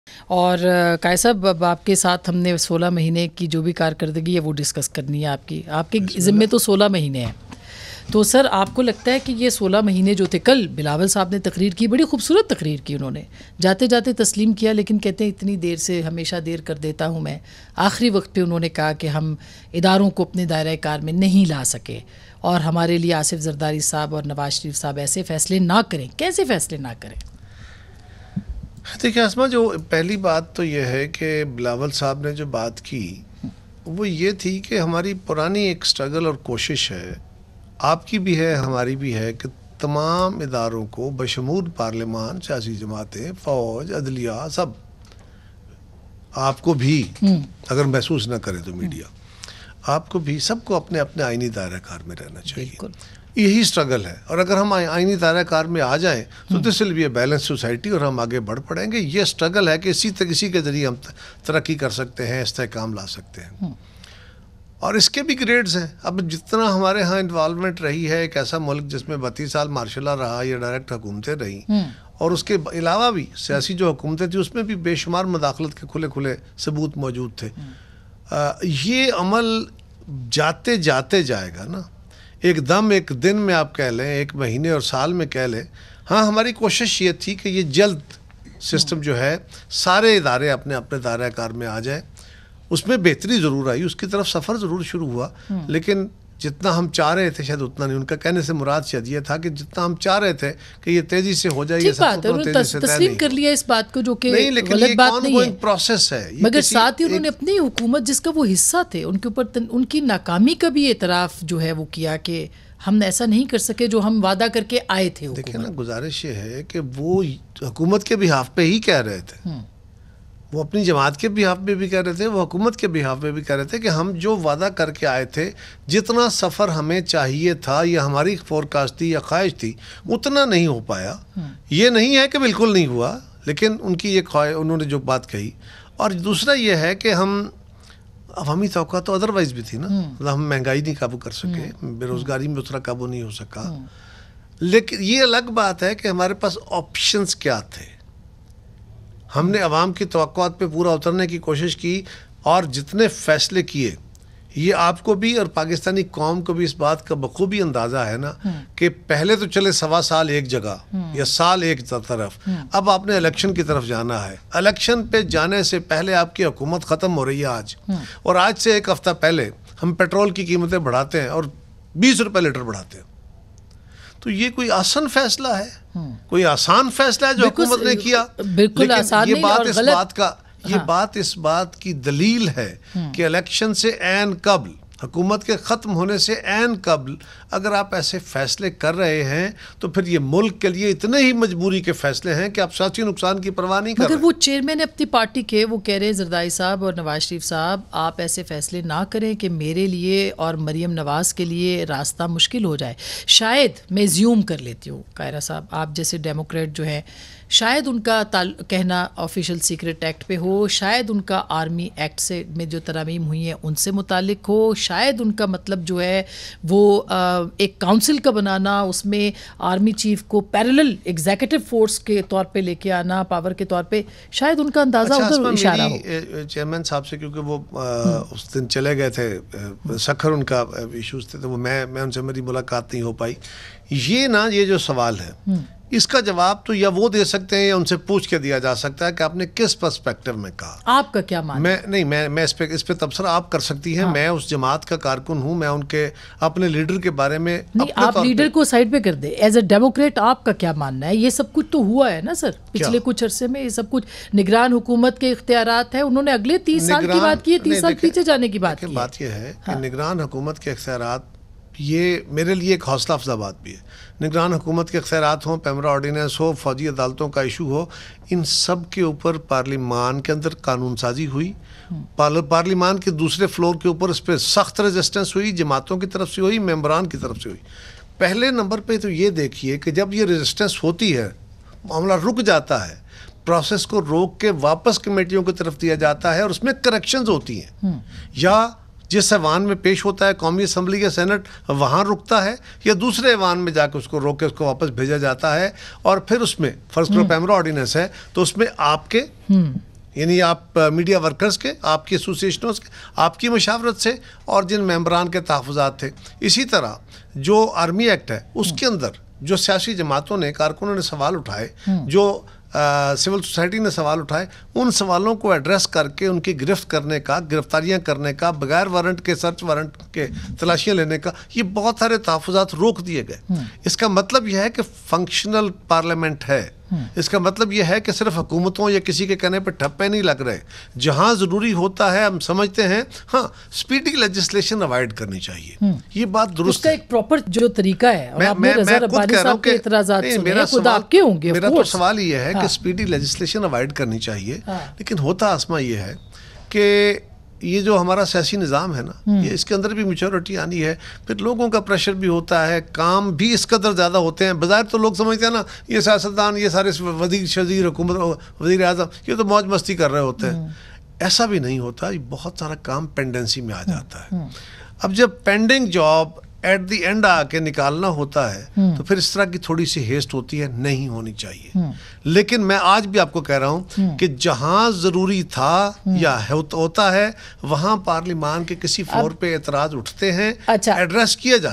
Did Bilawal Bhutto acknowledge the failures of govt in his speech yesterday?| Aaj News
Guests:
- Ather Kazmi
- Qamar Zaman Kaira

Aaj TV Official:
shorturl.at/iBDNX